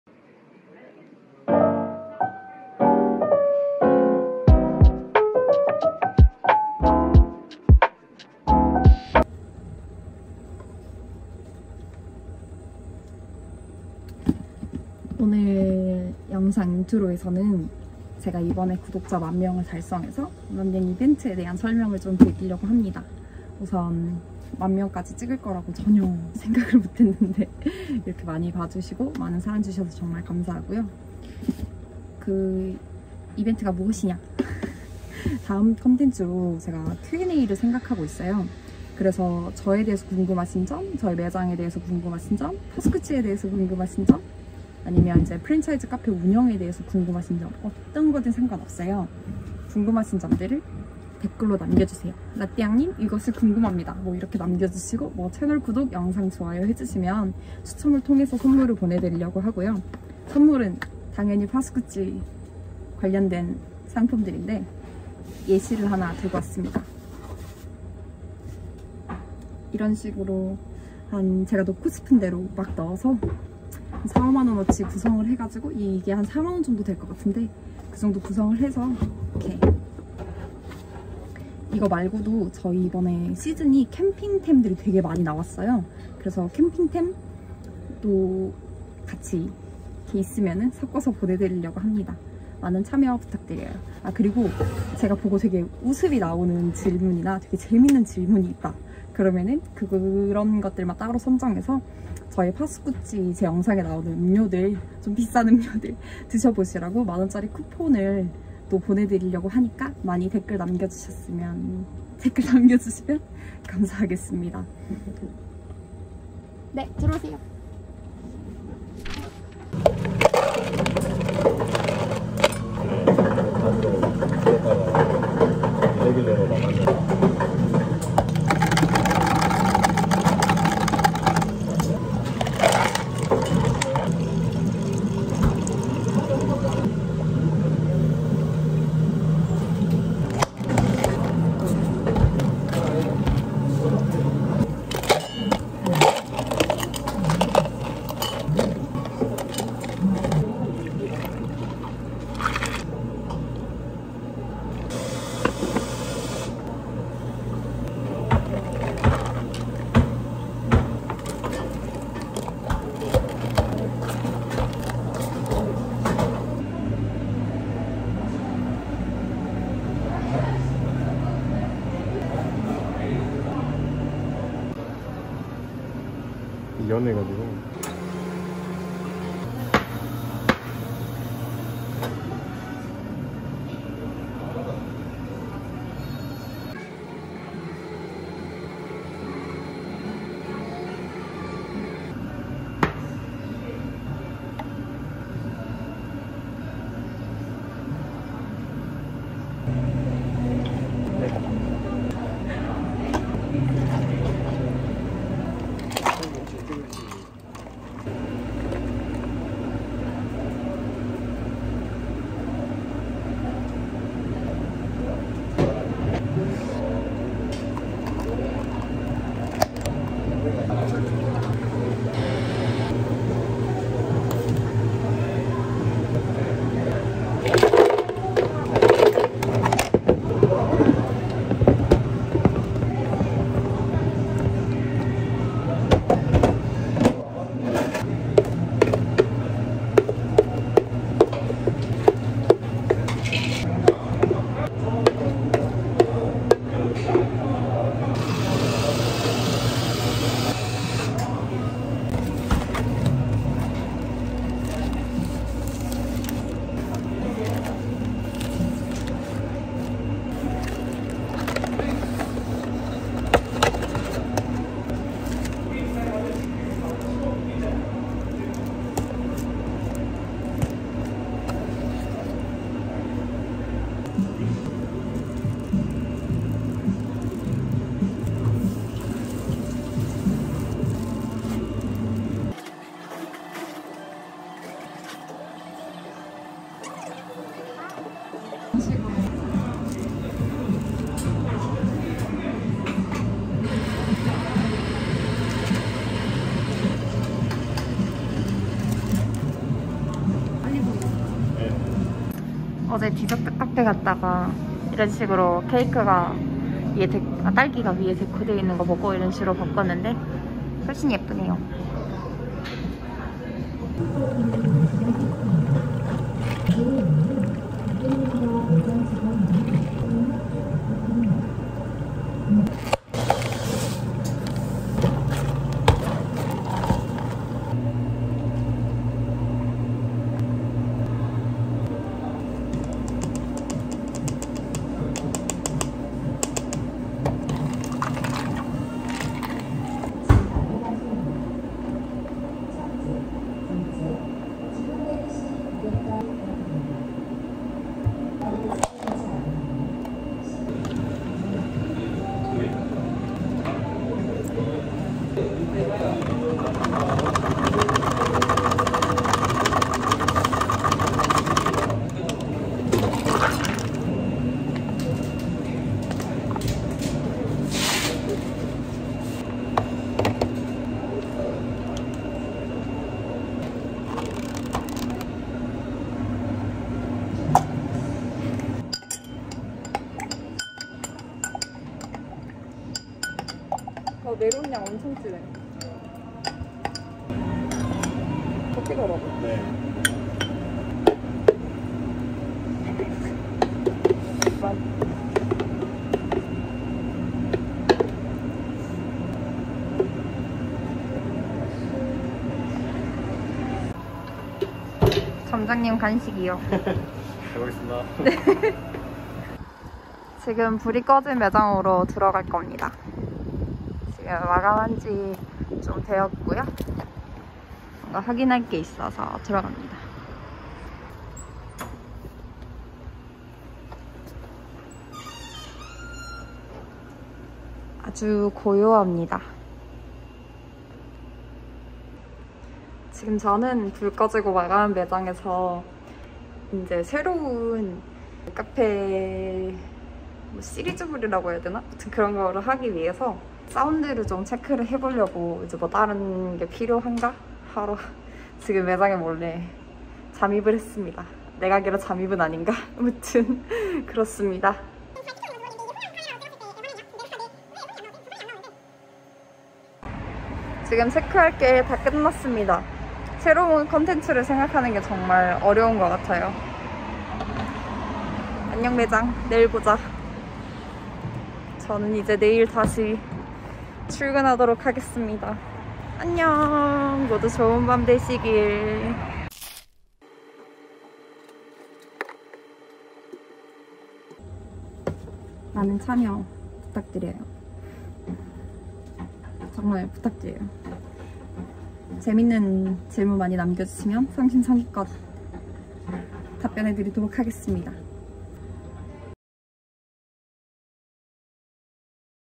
Eng) 카페 사장이 쏩니다!🔫 선물주는 카페브이로그,, 랍니다❣️/ CafeVlog
2. 라떼양에게 궁금한 질문을 댓글로 쓴다.
3. 설레는 마음으로 기다린다. 끝!

이벤트 기간은 2주 정도 진행할 예정이에요.
당첨자는 다음 Q&A 컨텐츠 업로드와 함께 발표하도록 하겠습니다.